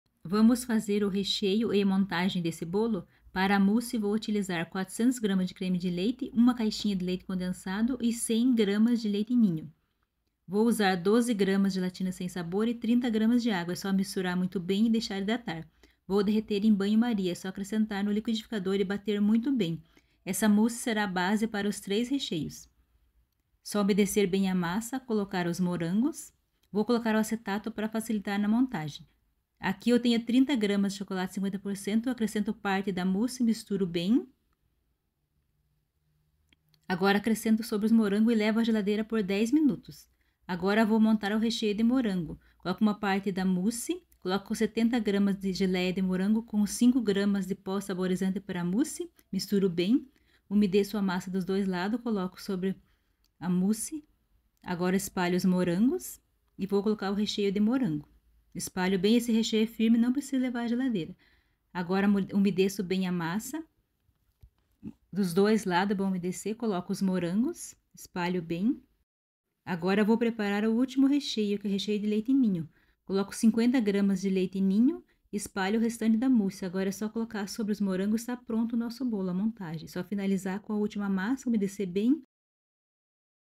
Receita do recheio e montagem do meu bolo @Elianekovalsk * receita na descrição do vídeo
ingredientes para mousse
400g de creme de leite ( 2 xícaras )
1 caixa de leite condensado ( 395g )
100g de leite ninho ( 1 xícara )
12g de gelatina sem sabor ( 2 c. de sopa)
30g de água ( 3 c. de sopa )

Modo de preparo está no vídeo

* Só dividir a mousse em três  partes iguais

Recheio de chocolate
300g de mousse ( 1 e1/2 xícara )
30g de chocolate 50% ( 3c. de sopa )

Recheio de morango
300g de mousse ( 1 e1/2 xícara )
70g de geleia de morango ou pasta saborizante ( 3 c. de sopa )
5g de pó para preparo de mousse ( 1 c.de sobremesa )

Recheio de leite ninho
300g de mousse  ( 1 e1/2 xícara )
50g de leite ninho  ( 5 c.de sopa )